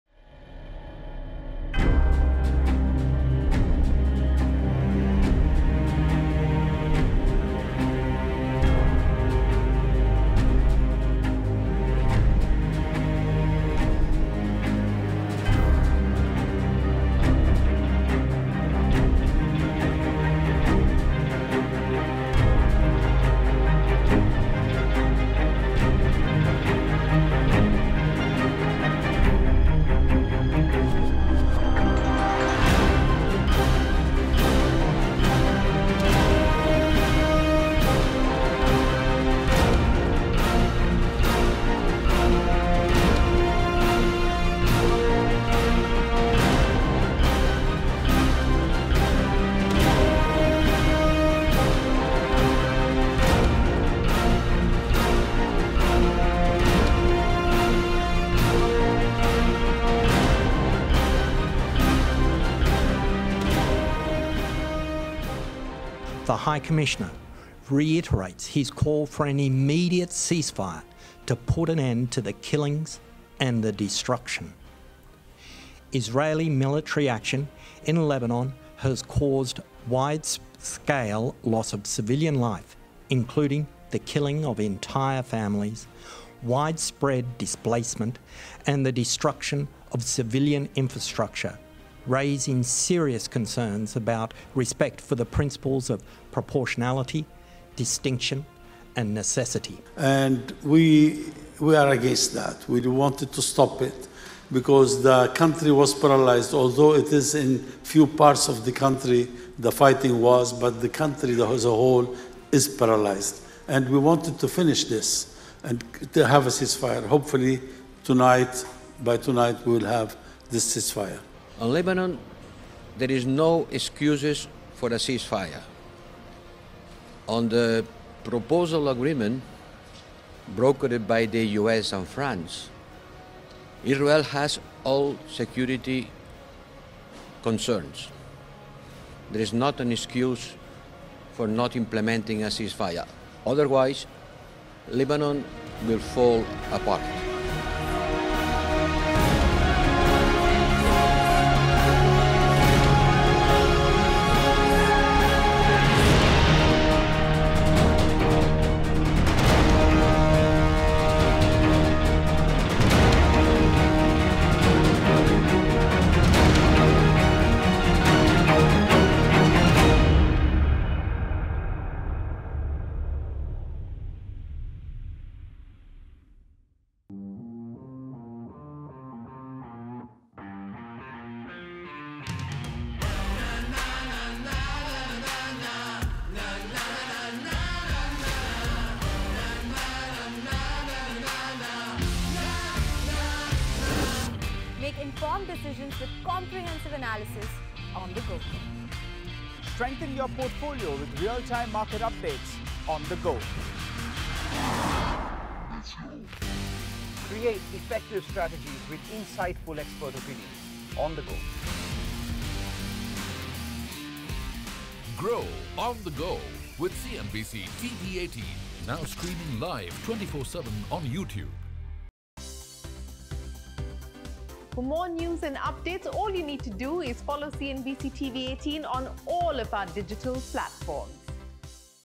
Ceasefire Deal Between Israel & Hezbollah Imminent: Sources | N18G | CNBC TV18
Israel & Iran-backed Hezbollah look set to reach a ceasefire deal on Nov 26. The agreement, negotiated by US & France, requires Israeli troops to withdraw from south Lebanon, while Hezbollah would end its armed presence along the border with Israel

n18oc_business